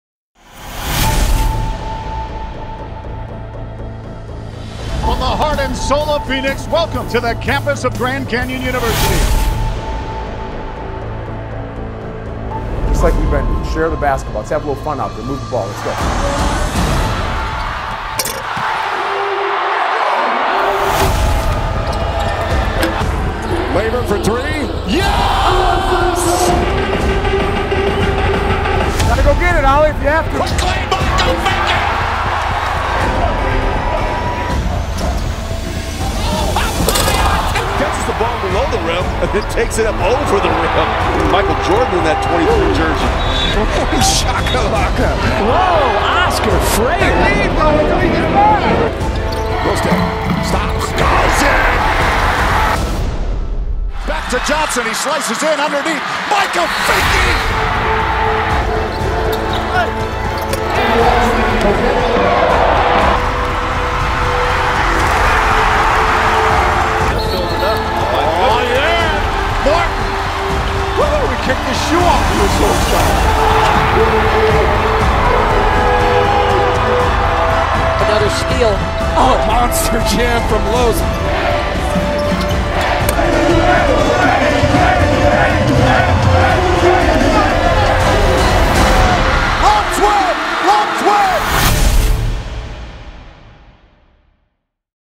GCU Men's Basketball 2018-19 Season Highlights
In a season that included another trip to the WAC title game, relive some of the best moments of the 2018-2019 season from the GCU Men's Basketball Team.